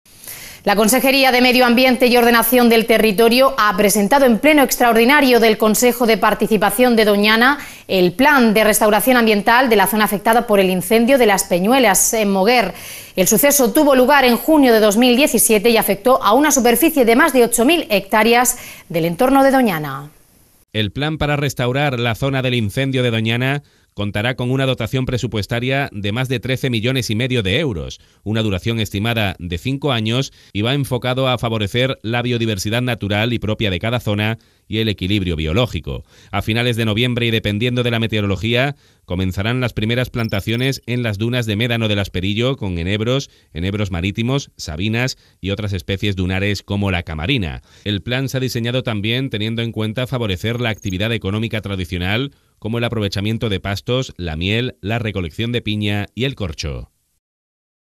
Noticia - El plan para restaurar la zona del incendio de Doñana contará con 13 millones
El plan para restaurar la zona del incendio de Doñana contará con una dotación presupuestaria de más de 13,5 millones de euros, una duración estimada de cinco años y va enfocado a favorecer la biodiversidad natural y propia de cada zona y el equilibrio biológico.

A finales de noviembre y dependiendo de la meteorología, comenzarán las primeras plantaciones en las dunas de Médano del Asperillo con enebros, enebros marítimos, sabinas y otras especies dunares como la camarina.

El plan se ha diseñado también teniendo en cuenta favorecer actividad económica tradicional, como el aprovechamiento de pastos, la miel, la recolección de piña y el corcho.

English:

The plan to restore the fire area of ​​Doñana will have a budget of more than 13.5 million euros, an estimated duration of five years and is focused on promoting the natural biodiversity of each area and the biological balance.

At the end of November and depending on the weather, the first plantations will begin in the dunes of Médano del Asperillo with junipers, maritime junipers, junipers and other dune species such as the camarina.

The plan has also been designed taking into account traditional economic activity, such as the use of pastures, honey, pineapple harvesting and cork.

Live streaming